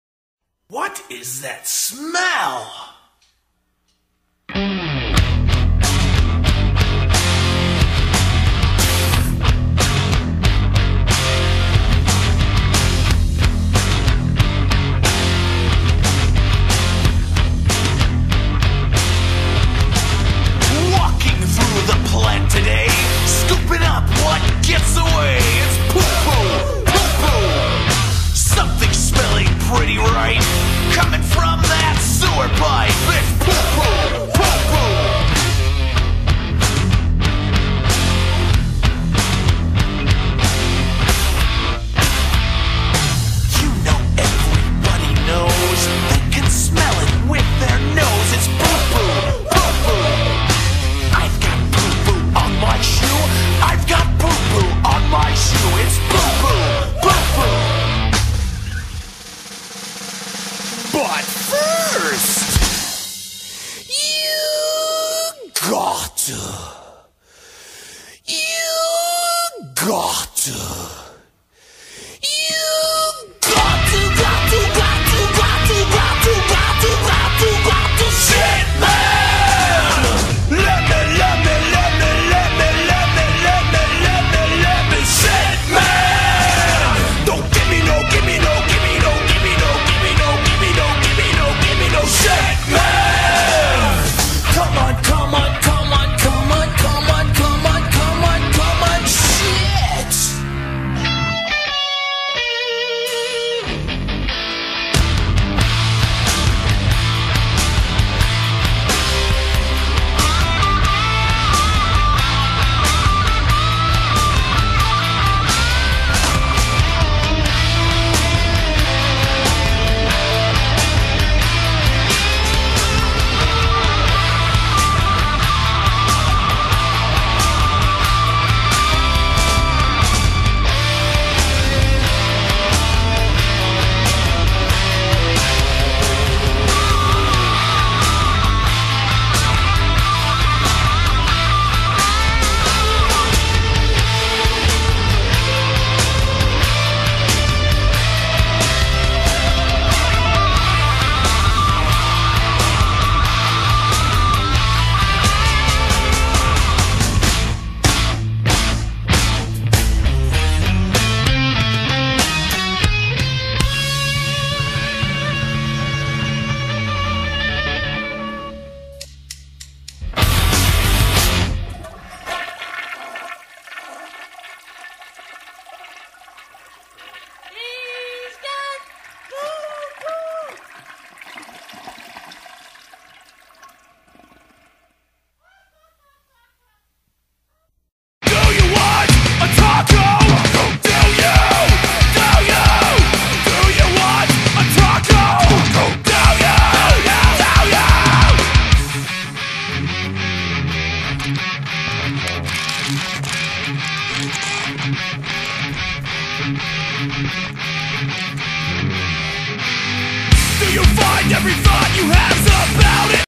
Nikkos MoSHIT
I created this video with the old status Windows Movie Maker for Windows ME...IT WAS HELL!But my video came out fine in the longrun. THIS IS WHAT HAPPENS WHEN I GET BORED! Enjoy this SHIT! I DID NOT CREATE THE MUSIC IN THIS VIDEO. I am not copyrighting material. The credit of the songs goes to Green Jelly and Psychostick. Thank You